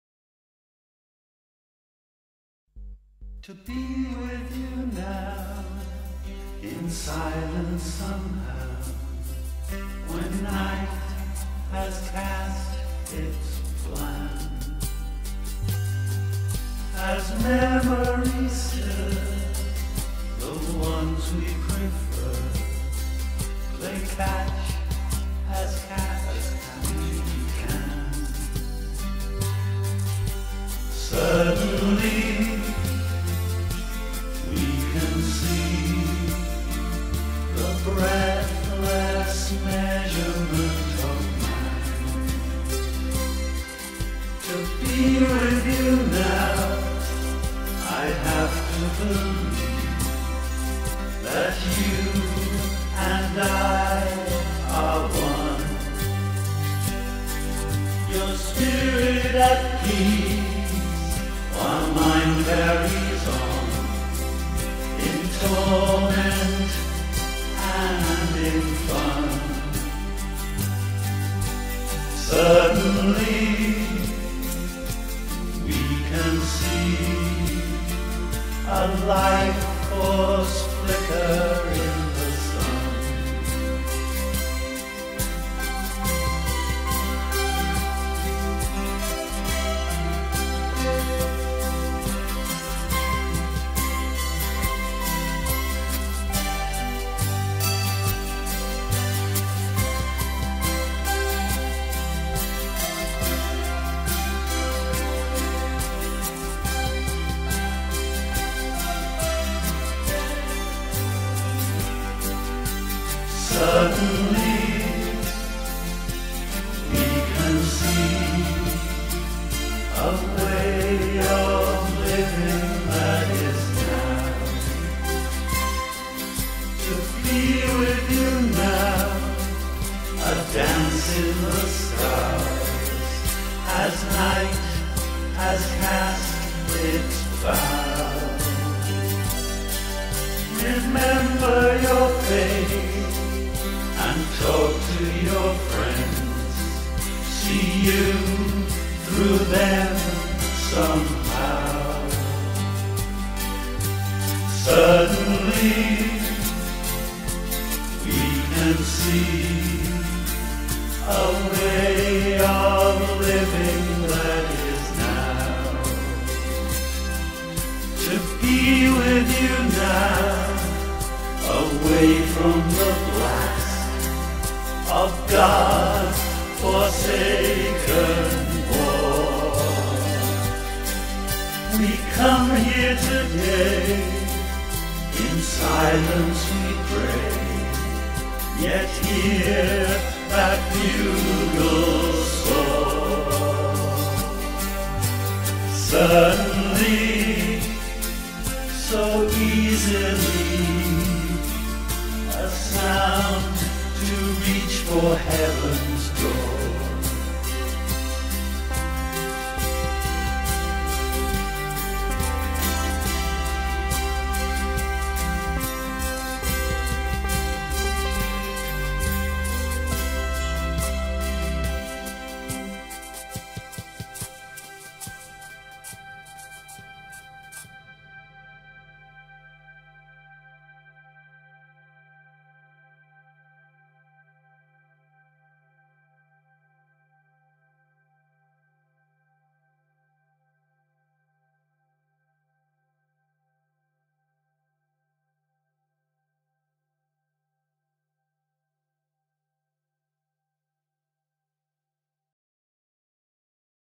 Requiem
Poet and Efran Kaye's tribute to everyone killed in wars.